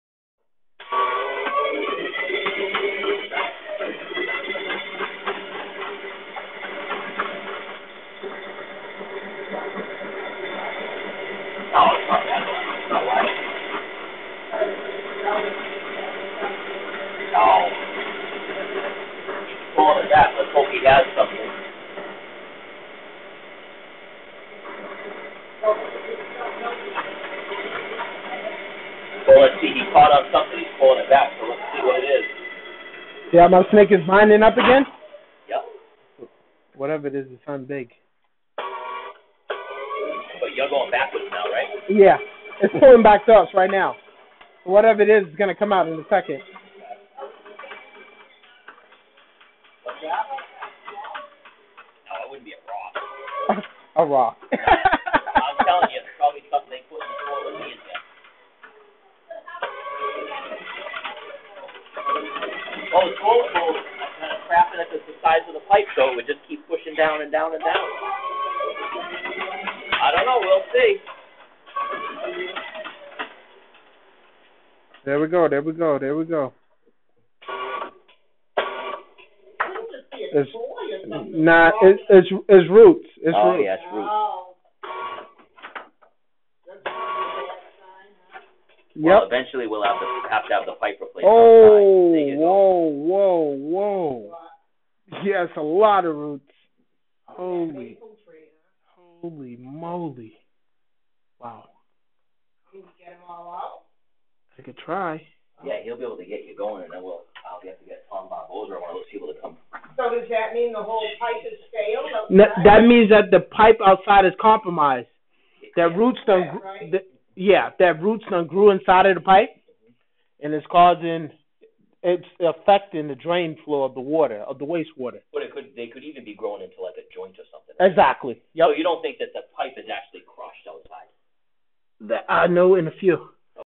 Clogged Drain full of Roots
Snaked this drain and it turns out it was full of roots!

Roots fees off of waste